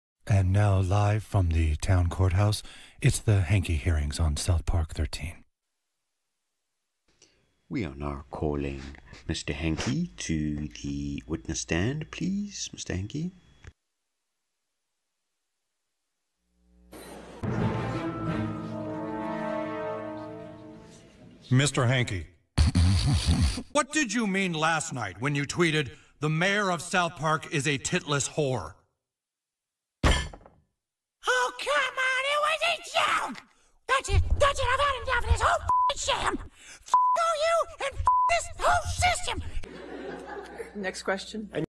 Anger in the court while witness is questioned
My take on the Johnny Depp and Amber Heard trial. This would have been a key witness!